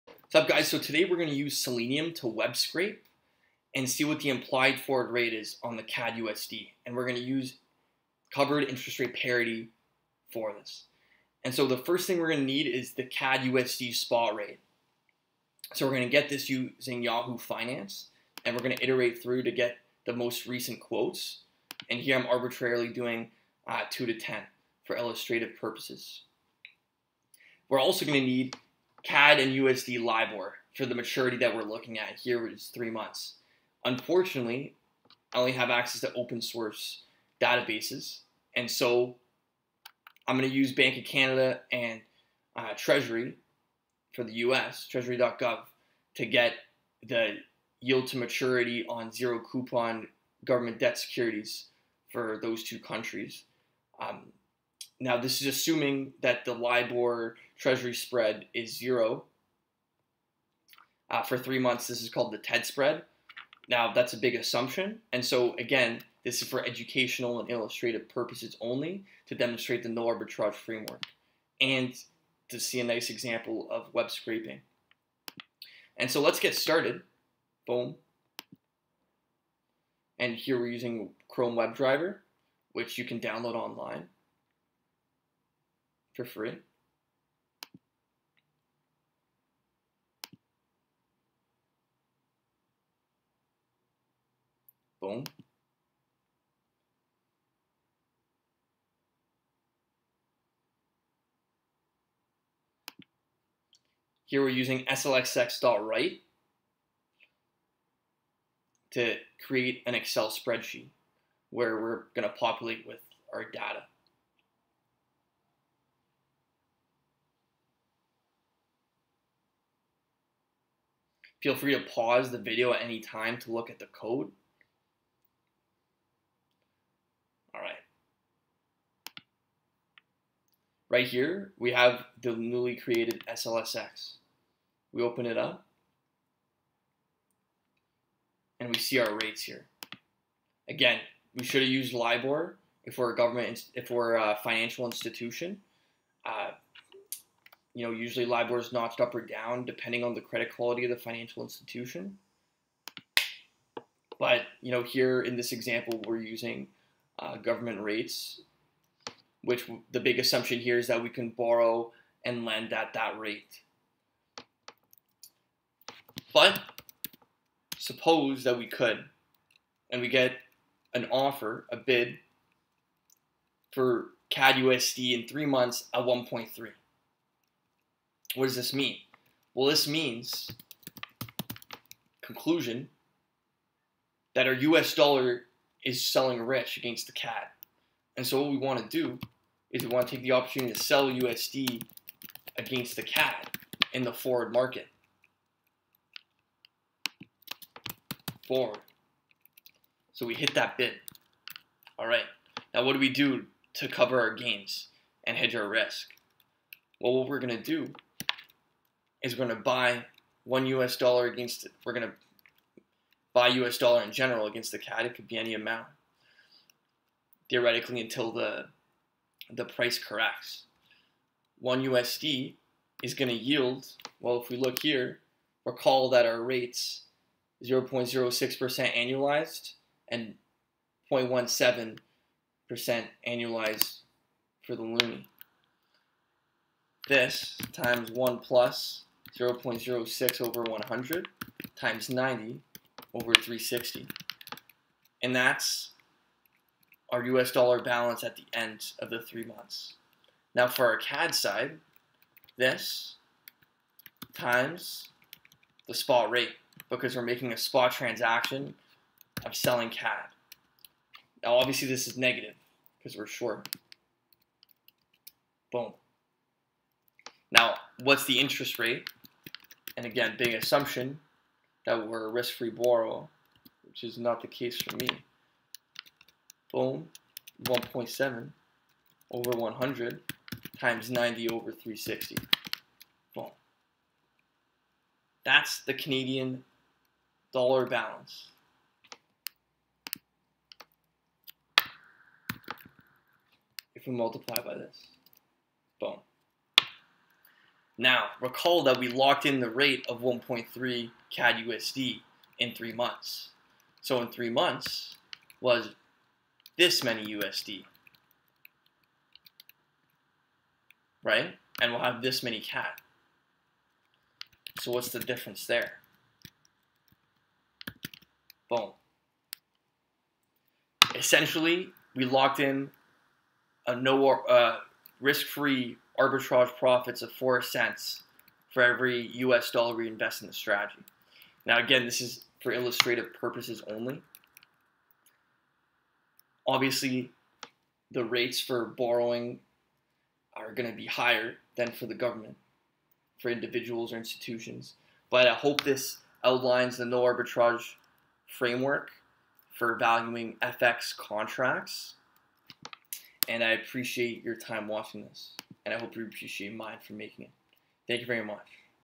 Python FX Forward Pricing Using Covered Interest Rate Parity
Note: I should have used USD and CAD 3m LIBOR but the ICE website uses captcha.